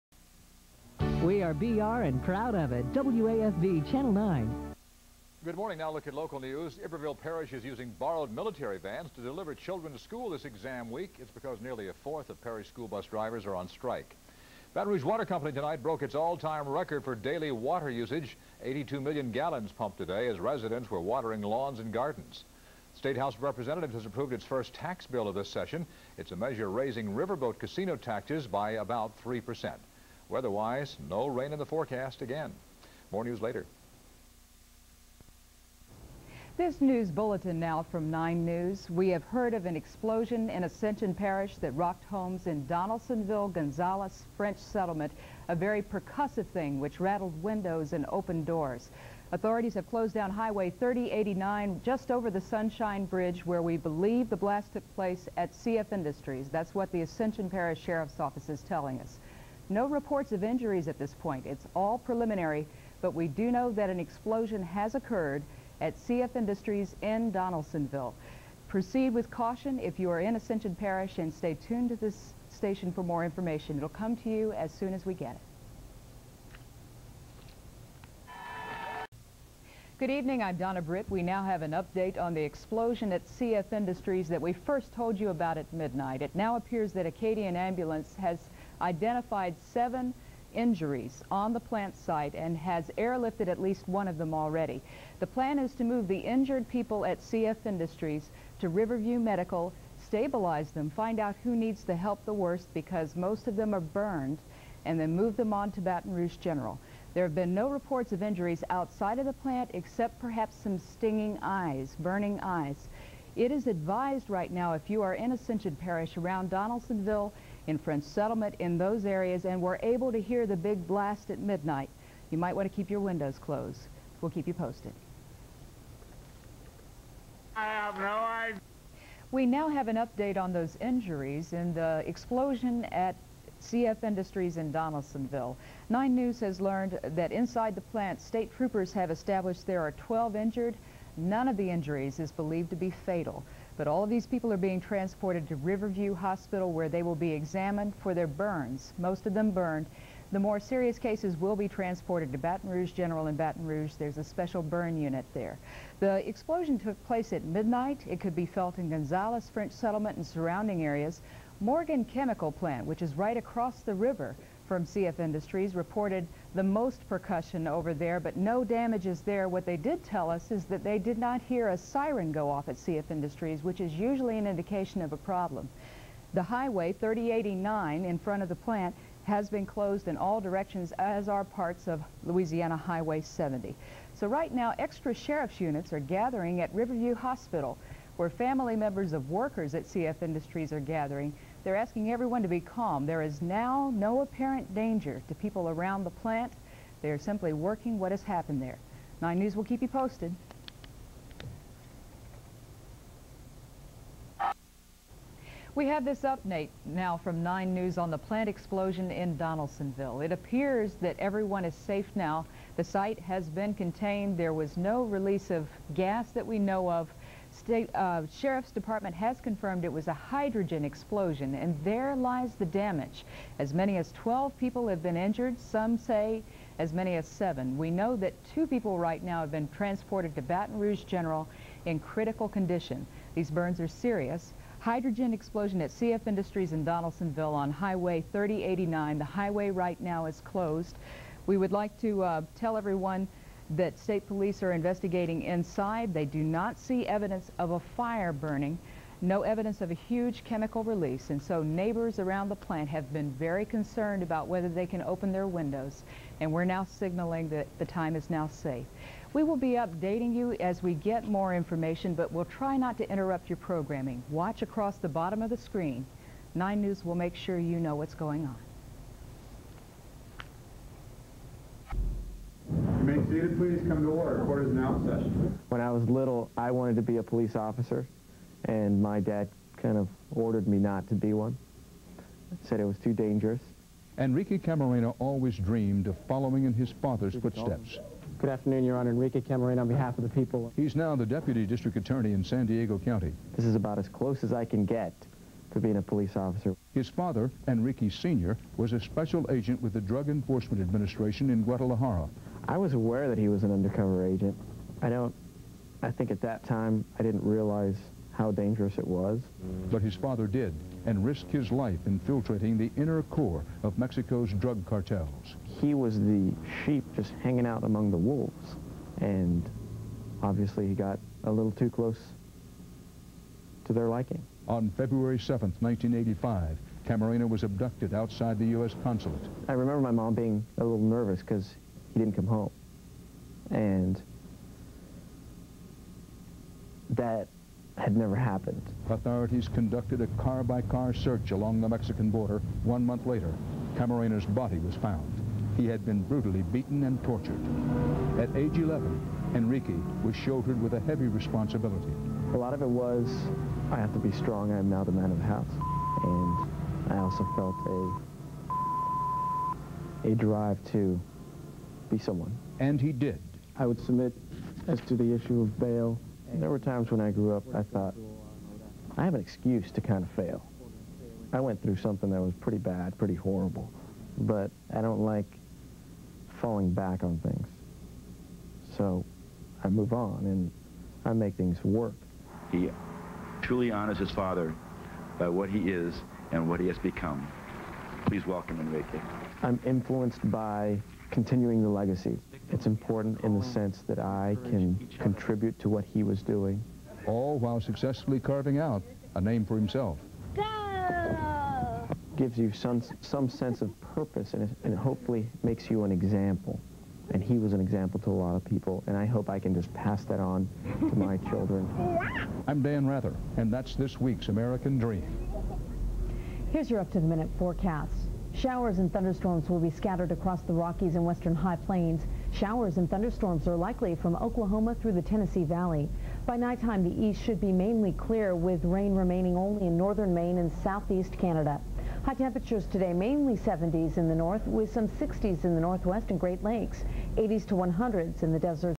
WAFB footage on CF Industries Explosion in Donaldsonville, LA May 24-25, 2000
On May 24, 2000, there was an explosion at the C. F. Industries Ammonia Plant in Donaldsonville (south of Baton Rouge).  This footage aired on WAFB very late in the evening May 24 and early in the morning May 25 mostly featuring Donna Britt.  The station cut in during the Late Show with Craig Kilborn, and did a scroll during the overnight CBS news program.  Copyrights acknowledged.